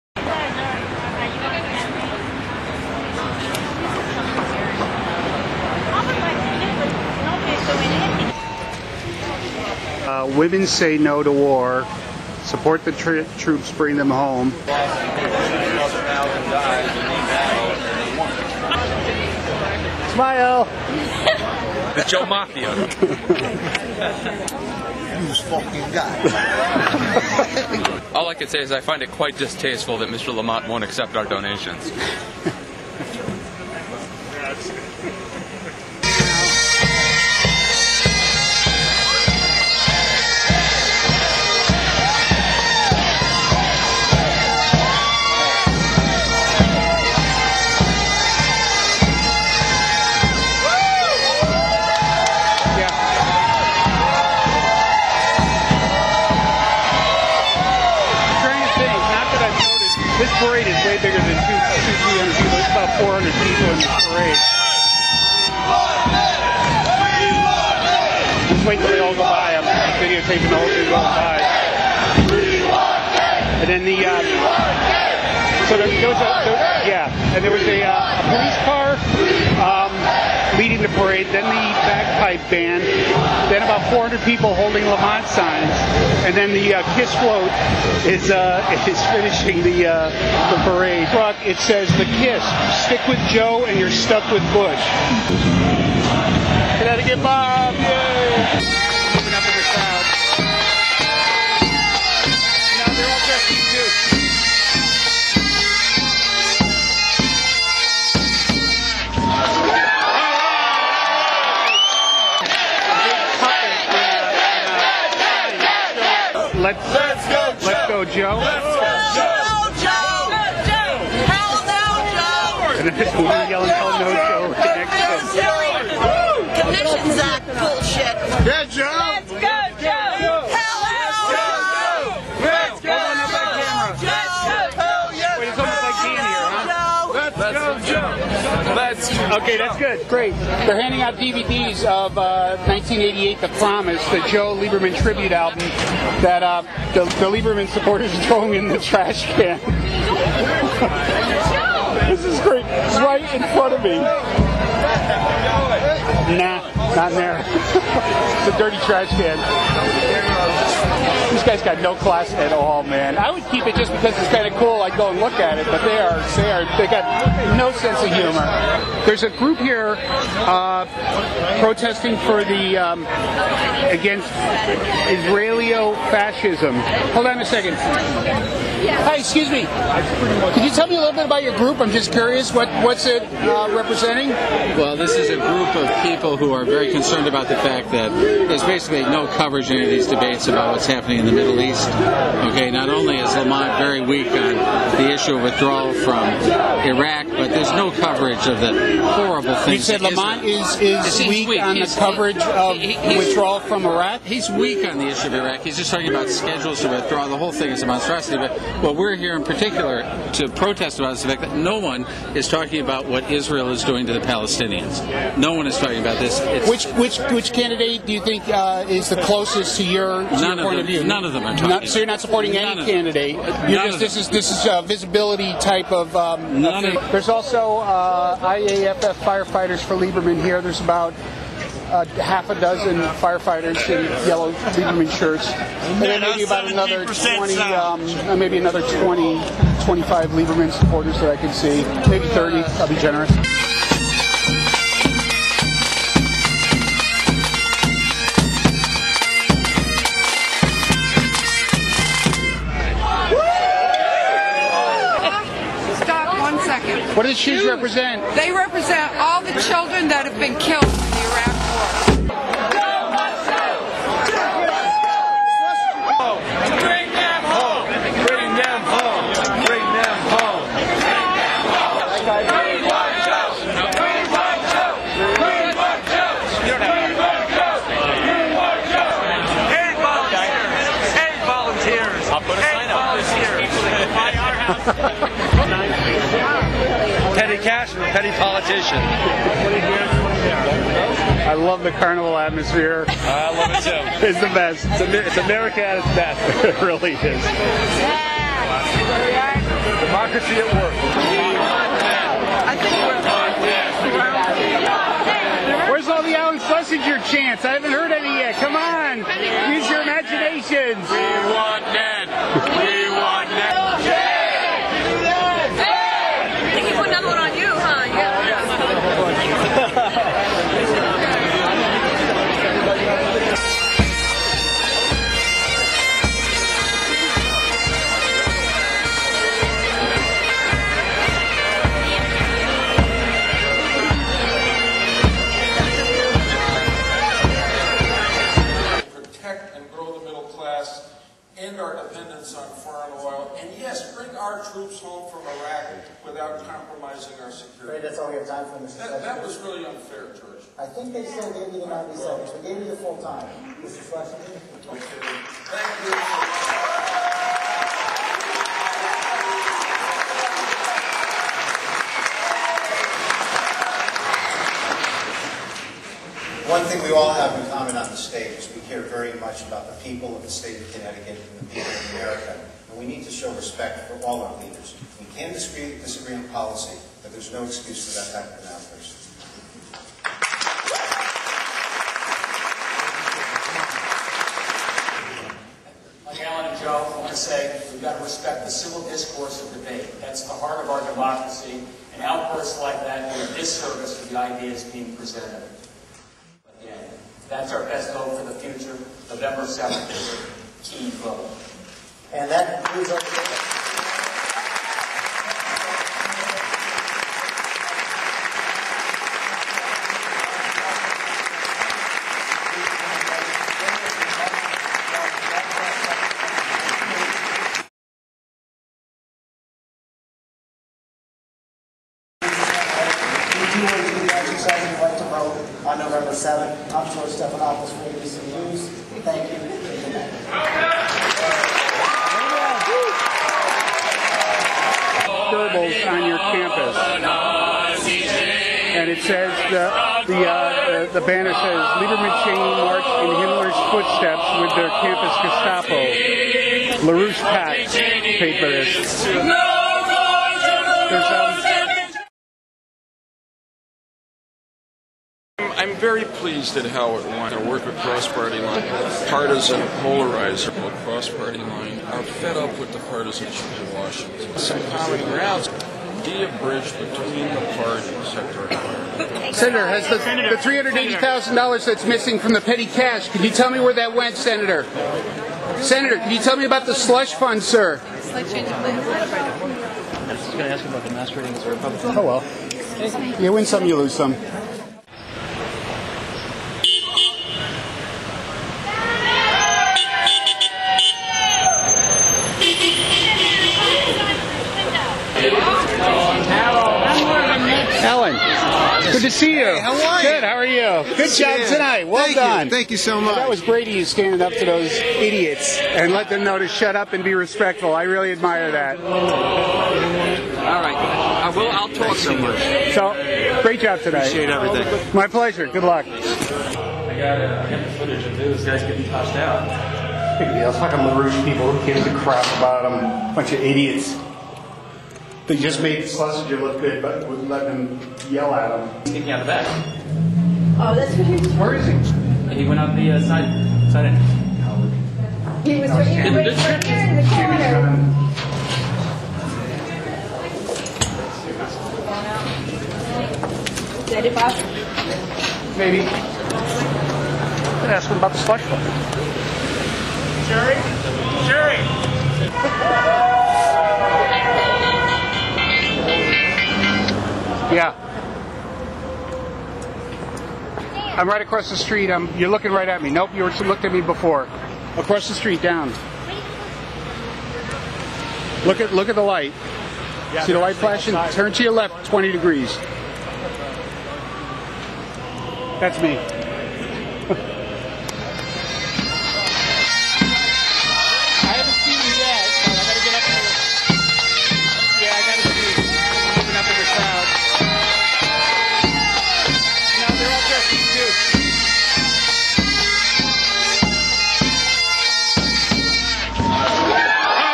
Impressions of the Debate
This is a mini-documentary about the CT Senate debate at the Garde Arts Theater in New London on October 23rd.  Don't mind the shaky camera, it's the content that counts.